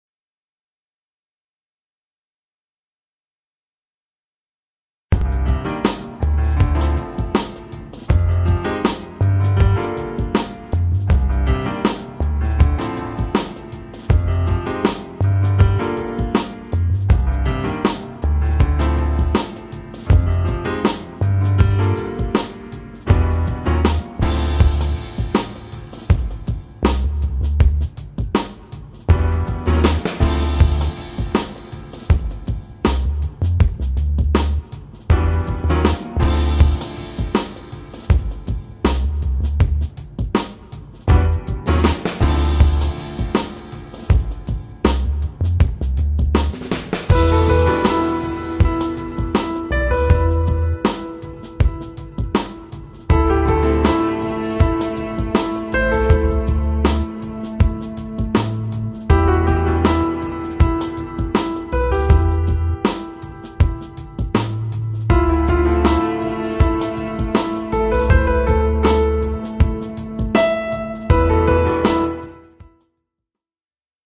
Renault clio occasion visible à Bourg en bresse présentée par Renault bourg en bresse
Cette Renault clio occasion disponible à Bourg en bresse dans le 01 est mise en vente par Renault bourg en bresse - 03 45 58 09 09 - tous les détails sont